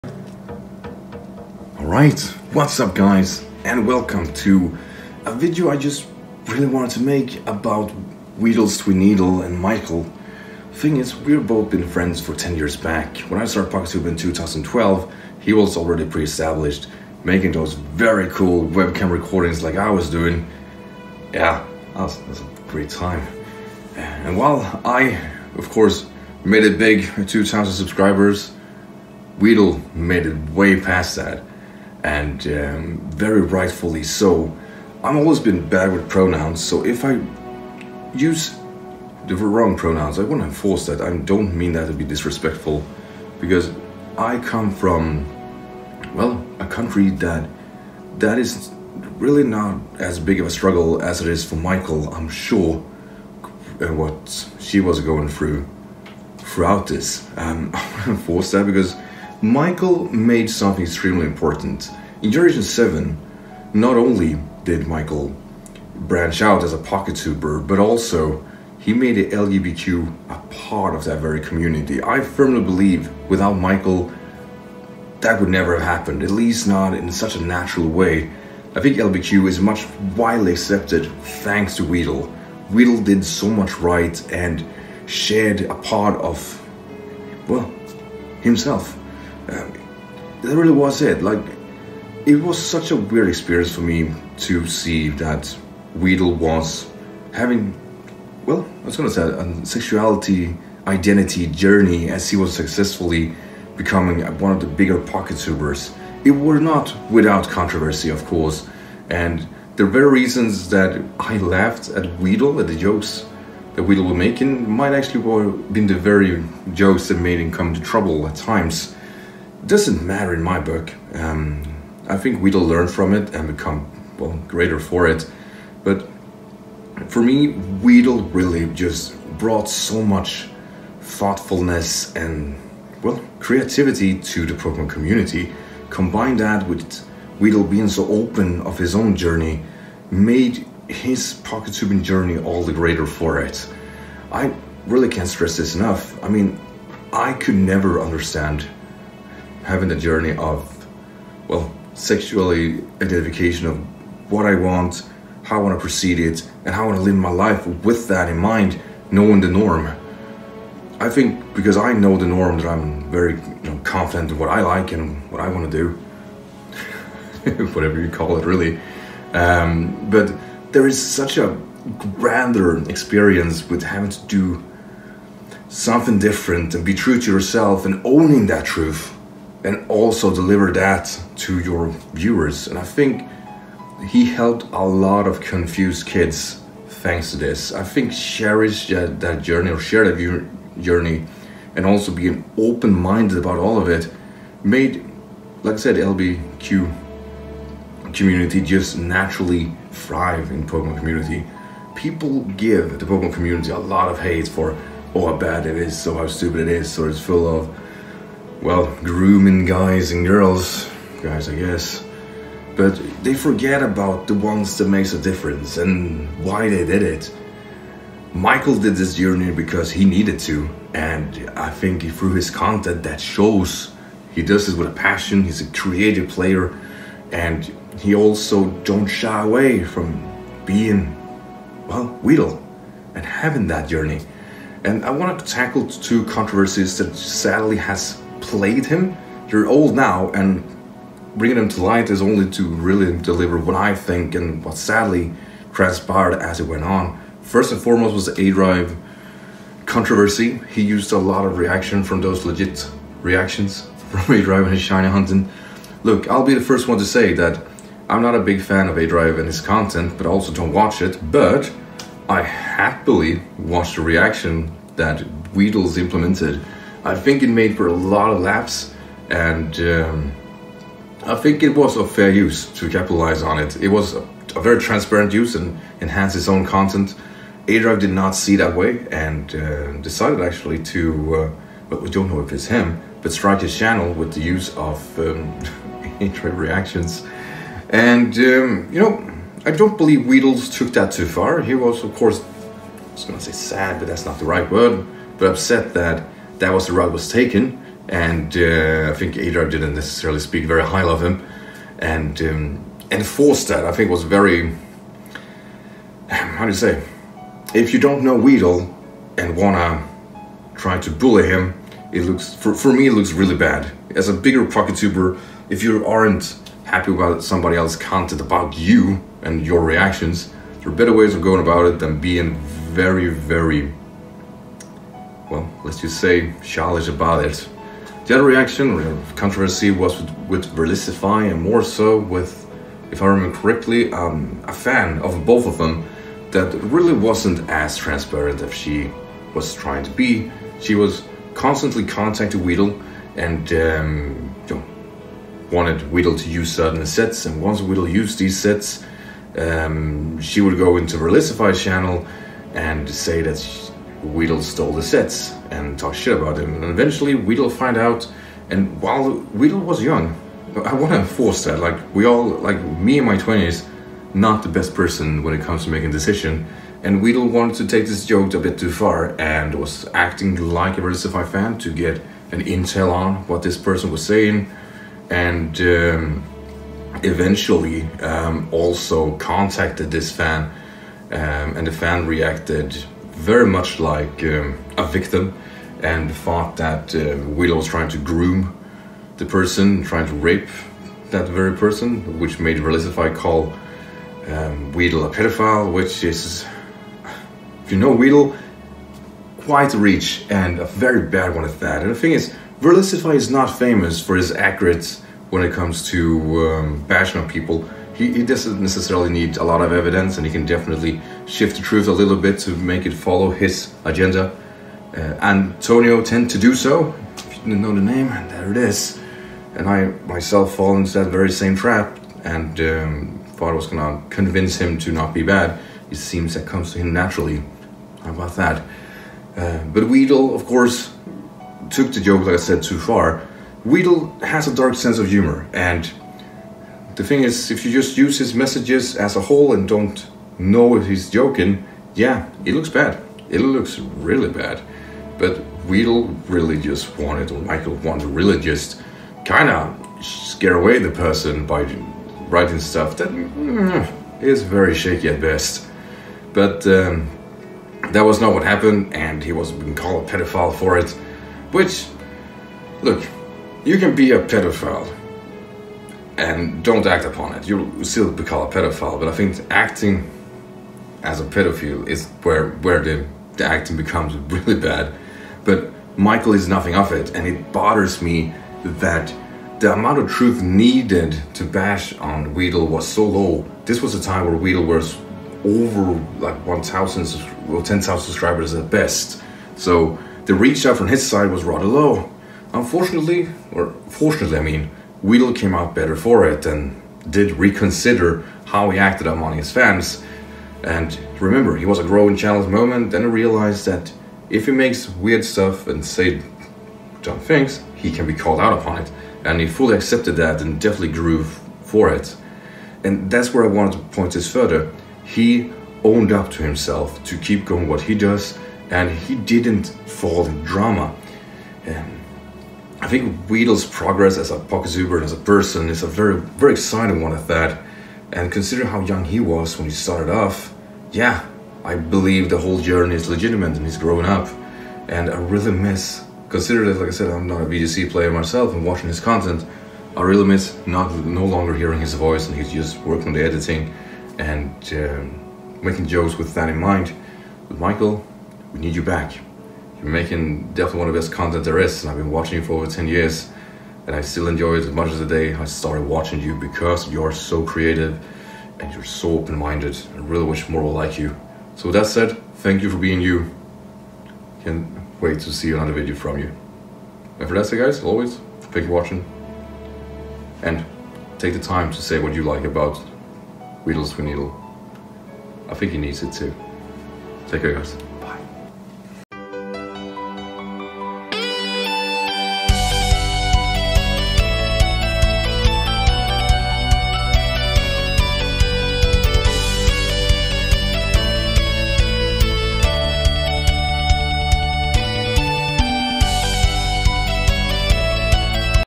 Why I watch WEEDLETWINEEDLE - Memes and LGBQ creation of a fantastic Poketuber
I watch Micheal and Weedles journey for over 10 years.
And is a person I simply enjoyed for what they brought to the Pokemon community.

I dont think anyone could be as open minded and so inviting like micheal has been since the channel blew up!
And we are all better thank to it!

Edit:,in this video I fail to call Weedle by theys pronanuce of they/them, and I wanna be as right as possible and sayvil sorry for failing to that in this video

▶️About Skyrander:
Im an old fart playing pokemon :D